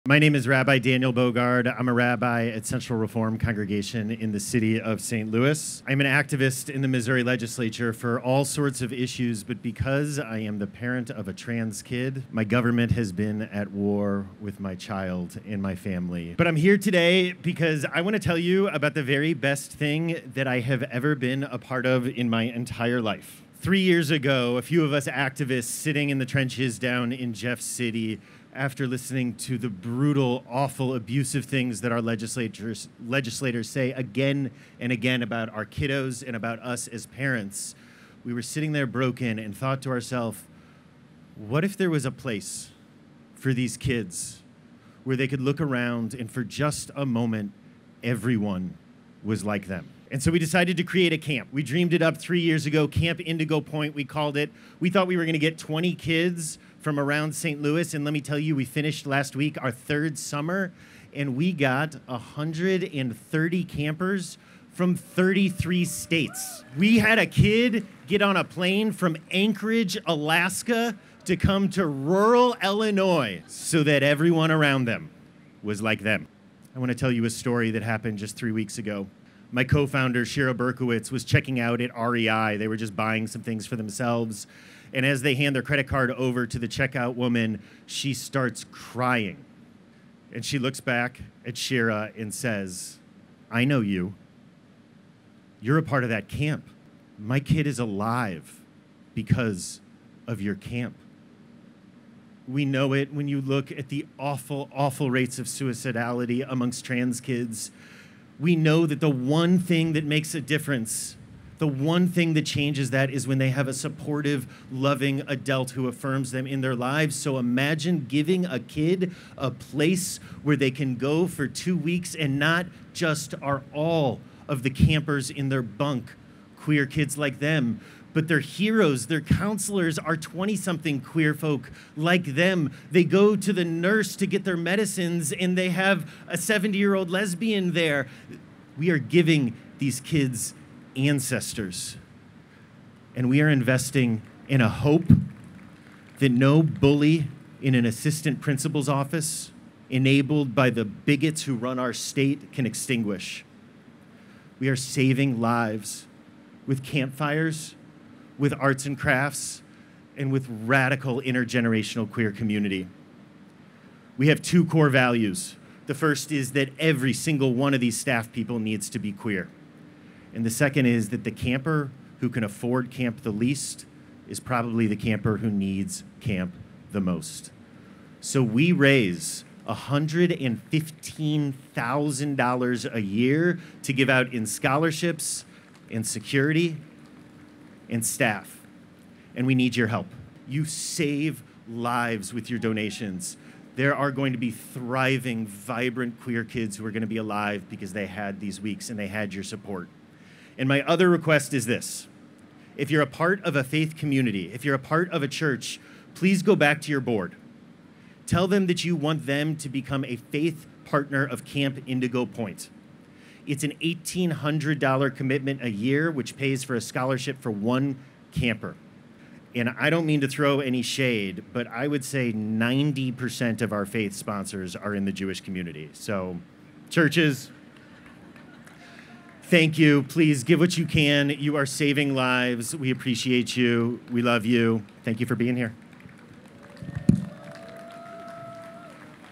STL Pride Interfaith Service: Offering for Camp Indigo Point
We're saving the lives of trans, gender expansive, and lgbtq youth with arts and crafts, campfire, and radical, intergeneration queer community. Please be a part of our work!